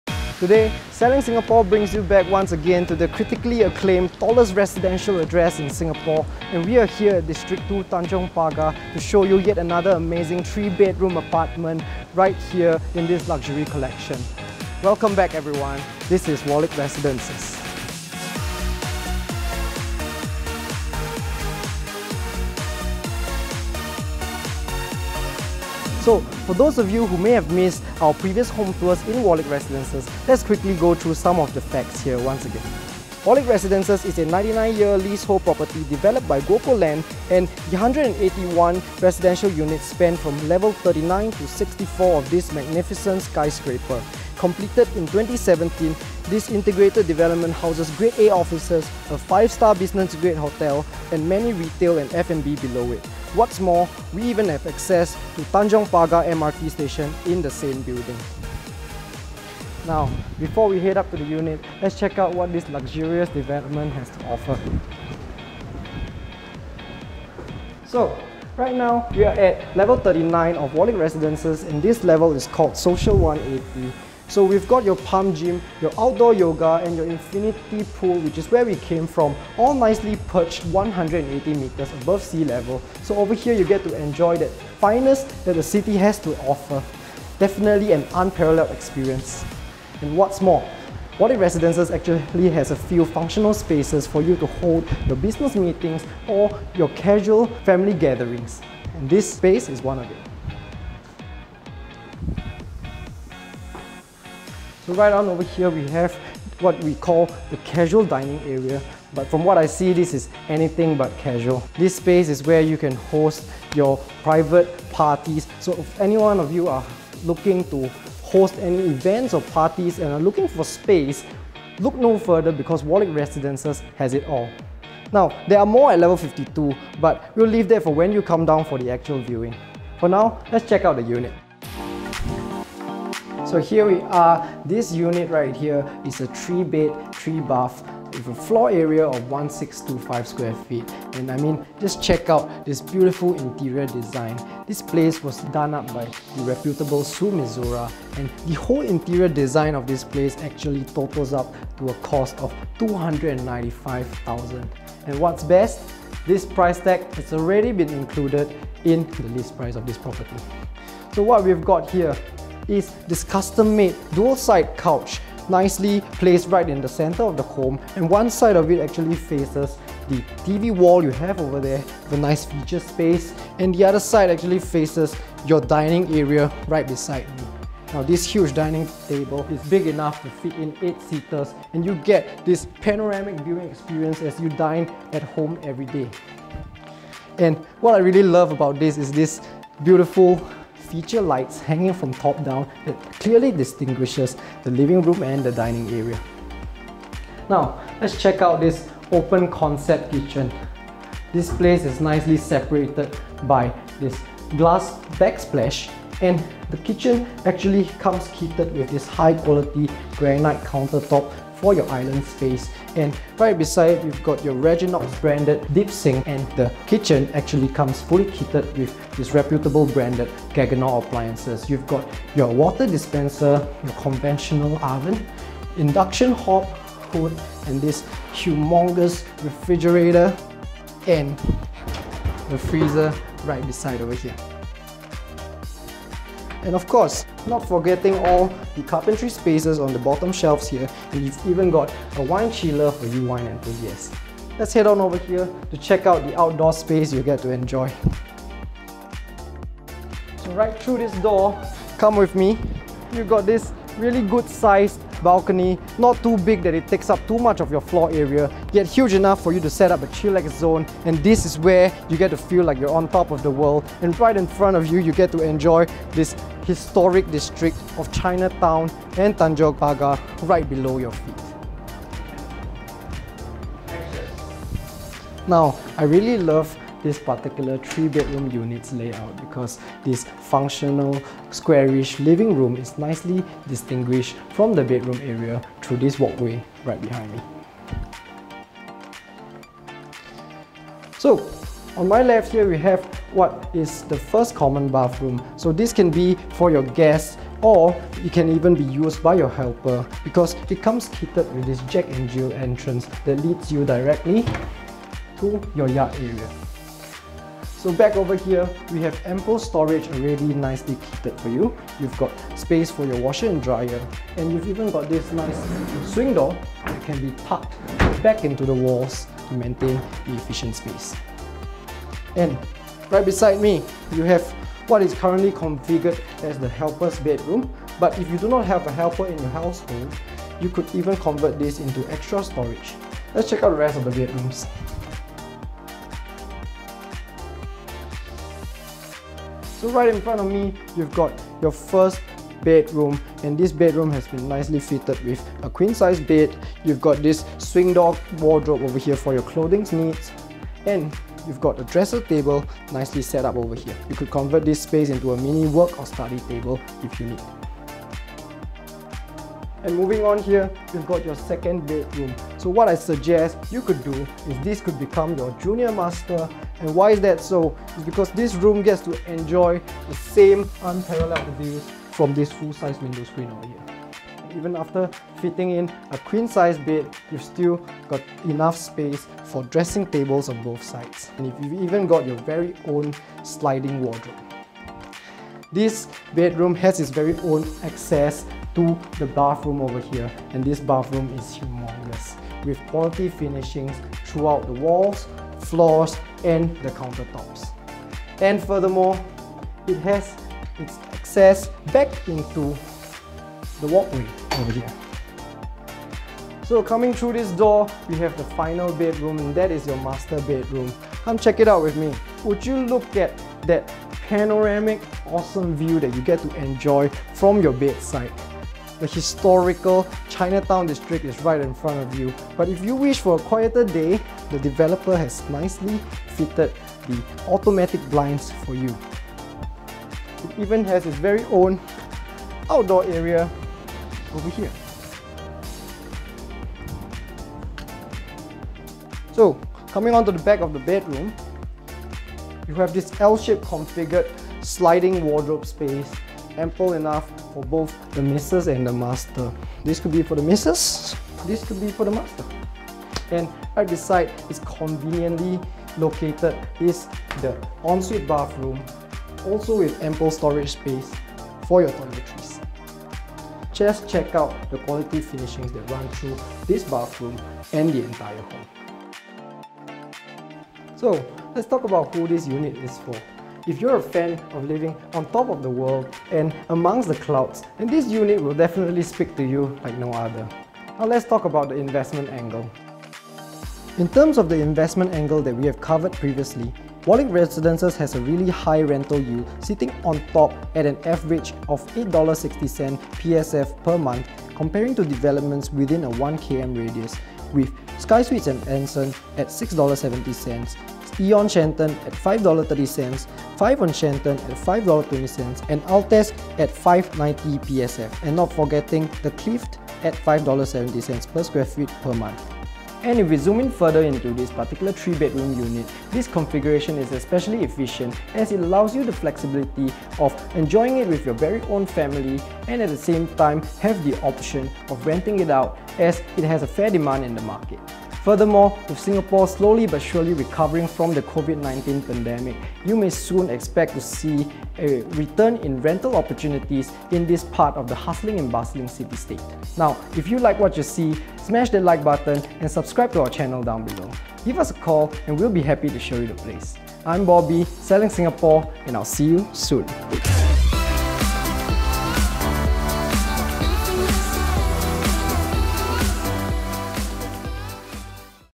SG Tallest Residential Address at Wallich ($5.53M) D02 Tanjong Pagar | Singapore Home Tour Ep.146
[New Listing]  One of the tallest condominium, Wallich Residence is a 99-year leasehold integrated development located at 3 Wallich Street in District 02. It houses Grade A offices, business hotel and extensive range of retail outlets. This project was developed by Guoco Limited, TOP-ed in 2018 and it stands 64 storeys tall and comprises a total of 181 units. It is within walking distance to MRT stations such as Tanjong Pagar, Telok Ayer, Downtown, Chinatown, Outram Park, Raffles Place, Marina Bay.

This unit features a 3-bed 3-bath, with a floor area of 1625 sqft.

00:00 [Intro]
01:07 [Level 39]
02:20 [Living Room]
02:53 [Dining Area]
03:17 [Open Concept Kitchen]
04:10 [Balcony]
04:54 [Common Bathroom 1]
05:08 [Storage Space]
05:22 [Helper's Room]
05:39 [Bedroom 1]
06:00 [Junior Master Bedroom]
06:25 [Common Bathroom 2]
06:49 [Master Bedroom]
07:37 [Ensuite Master Bathroom]
07:50 [Who This Is For]
08:00 [Investment Angle]
09:00 [Outro]

If this feels like home and we'd love to show it to ya...